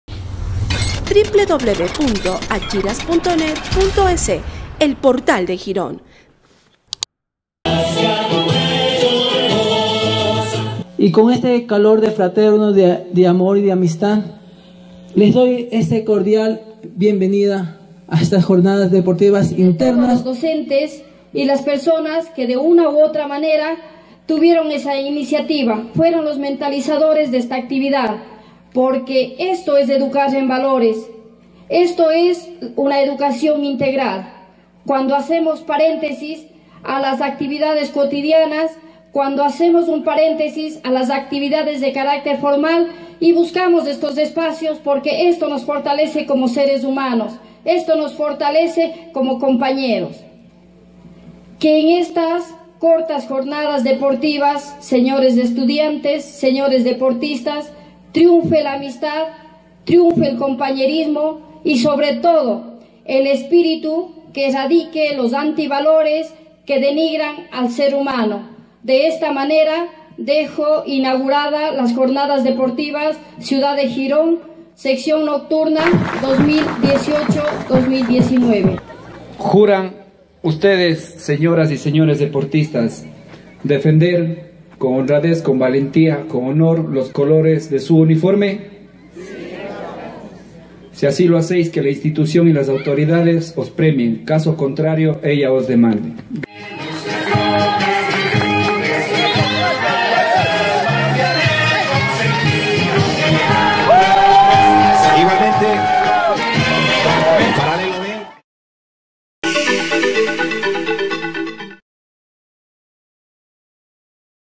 Sección nocturna de la UE Ciudad de Girón tuvo sus jornadas deportivas internas
En la noche del 11 de octubre se desarrolló la Inauguración de las jornadas deportivas internas de la Unidad Educativa Ciudad de Girón sección nocturna dentro del proyecto de educación básica y bachillerato intensivo para jóvenes y adultos con escolaridad inconclusa denominada  monseñor Leonidas Proaño. El acto inaugural y juegos se cumplieron en las instalaciones del antiguo colegio Rafael Chico, y se desarrollaron durante los días jueves y viernes. Quedaron como campeones en la categoría damas el paralelo de bachillerato y, de varones el paralelo “A” de básica. Les presentamos un reporte del vicerrector de la institución, Flavio Villa.